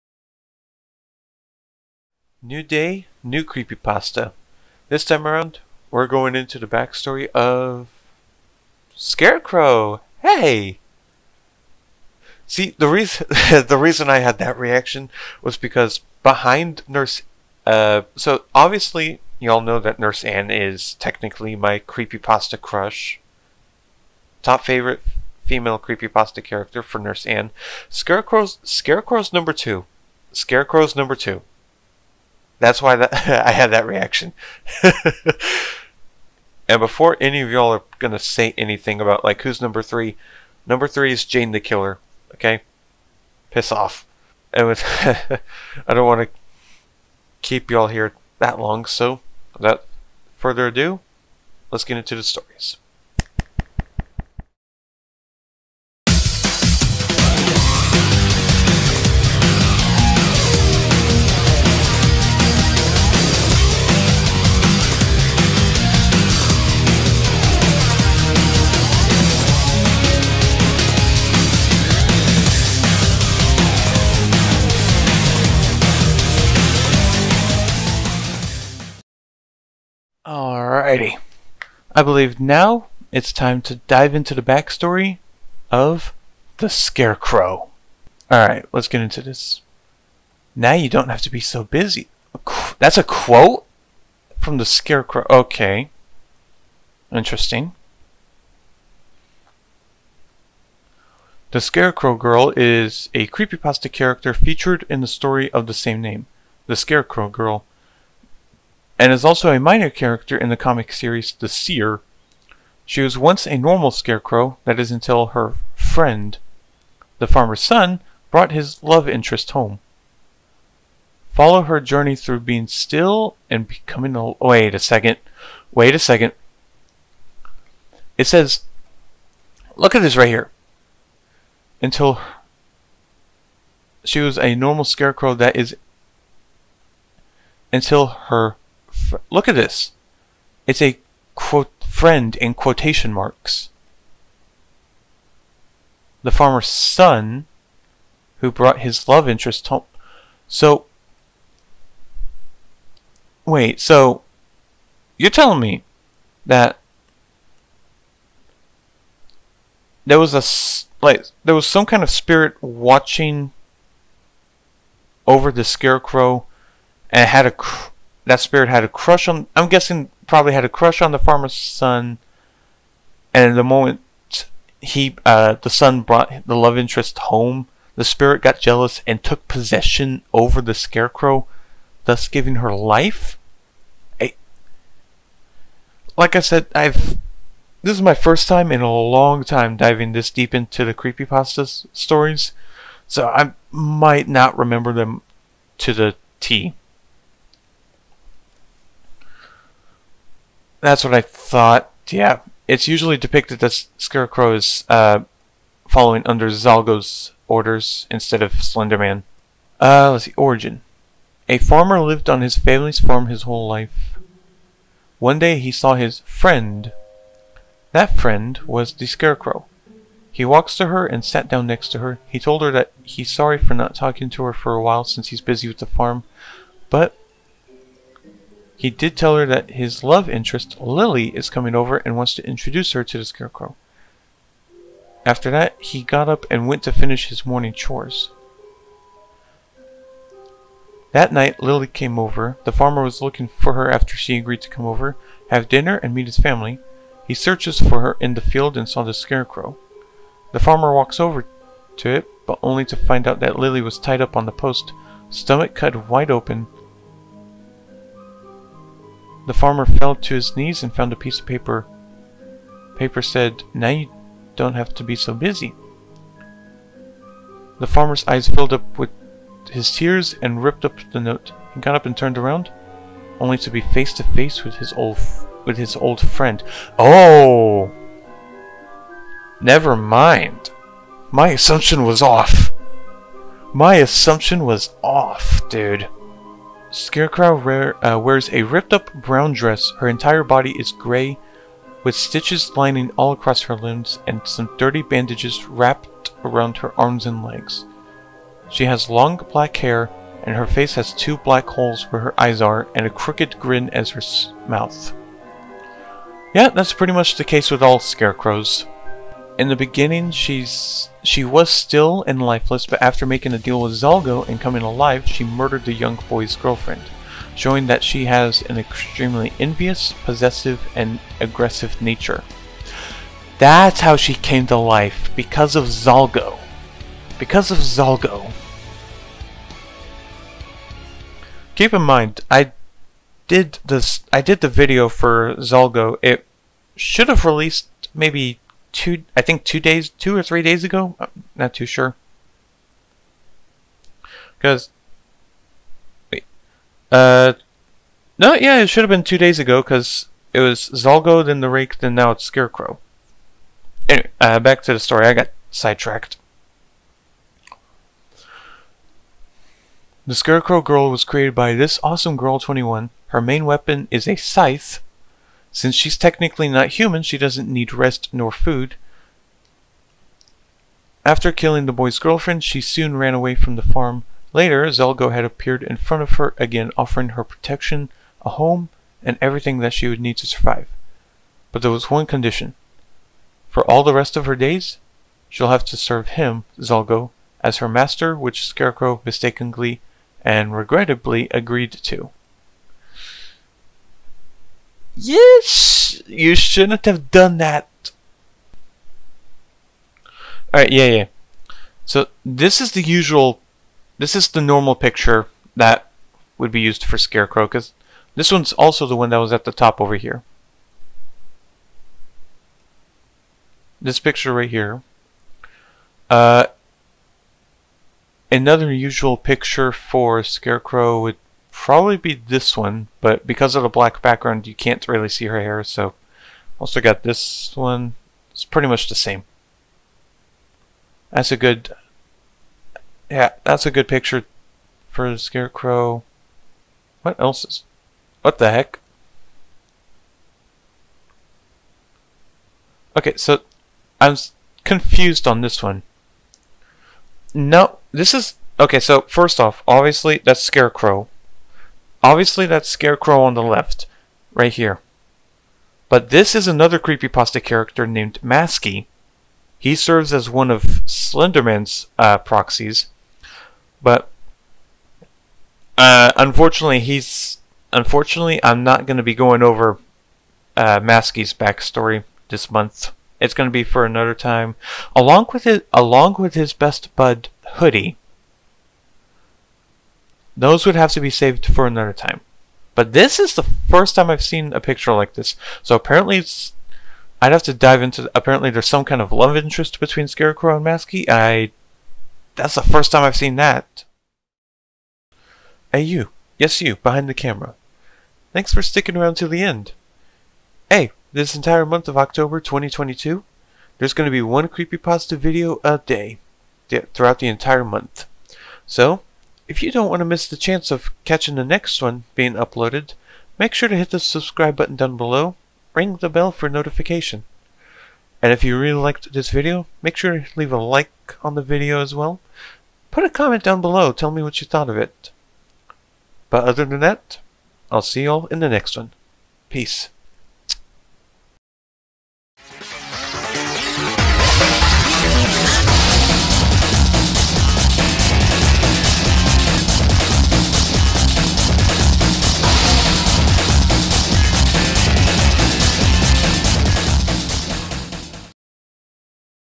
Creepypasta October. Diving into the backstory of The Scarecrow
Today for Creepypasta October, we dive into the backstory of The Scarecrow Girl.